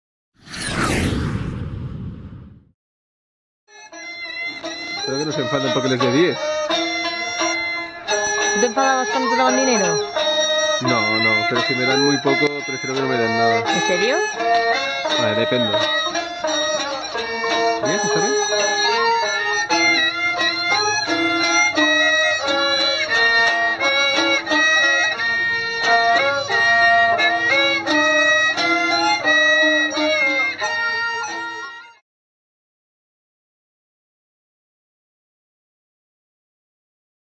Músico en Baisha
En el pueblecito de Baisha, en Yunnan (China), unos músicos tradicionales de la etnia Naxi me invitaron a tocar con ellos porque les había hecho un donativo.